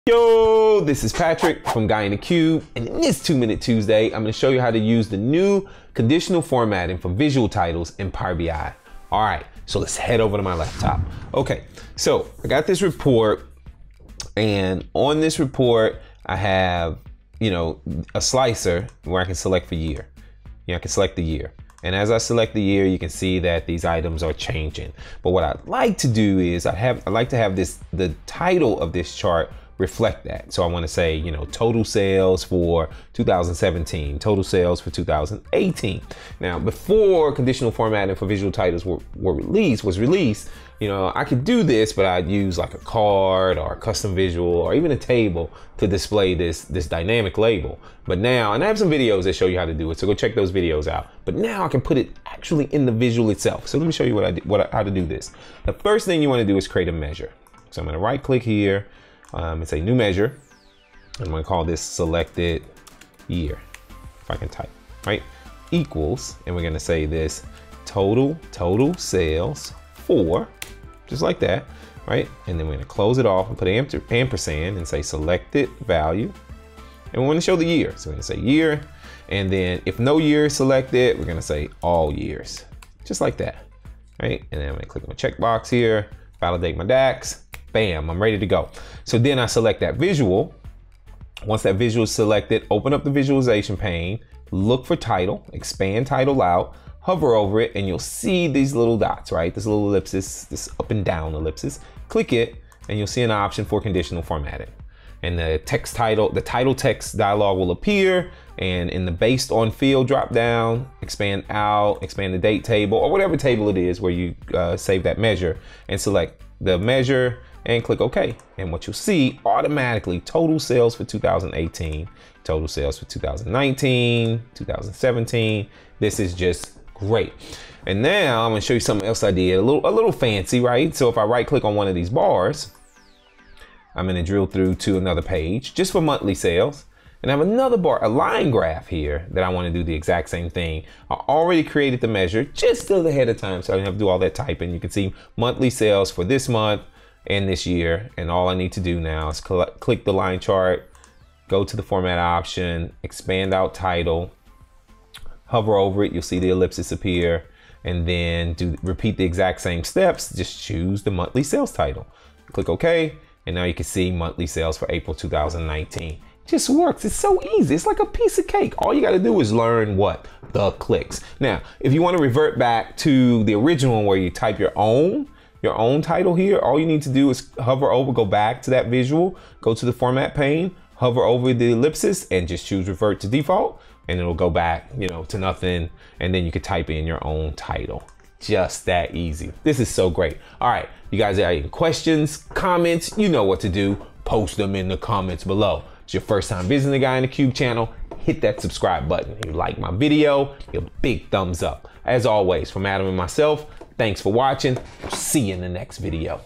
Dynamic Titles in Power BI Desktop
Patrick shows you how you can easily create dynamic titles in Power BI Desktop. This became available in the April 2019 release of Power BI Desktop.